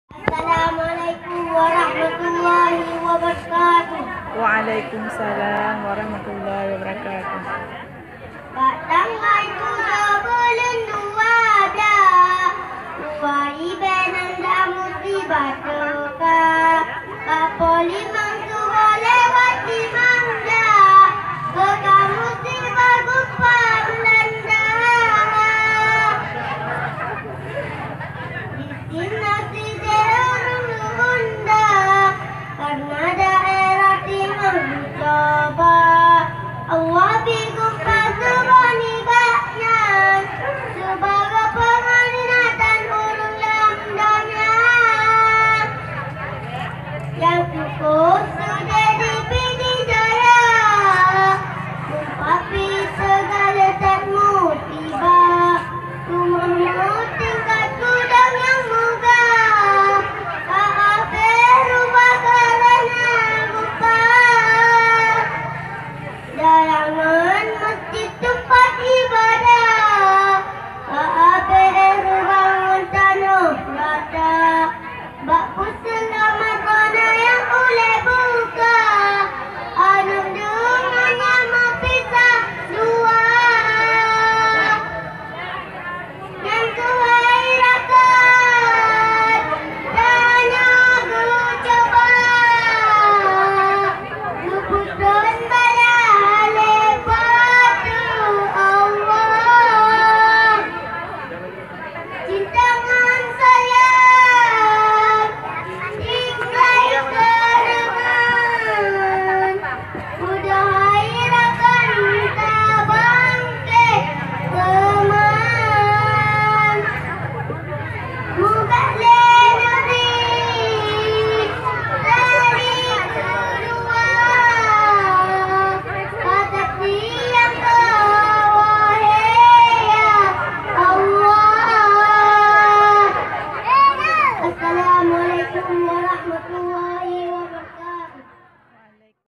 [TK Sirajul Huda] Lagu Gempa Aceh 2016
Aksi Peserta Didik TK Sirajul Huda pada Selingan Acara Lomba di SDIT Muhammadiyah Bireuen.

Mengenang Gempa Aceh 07 Desember 2016 di Pidie Jaya.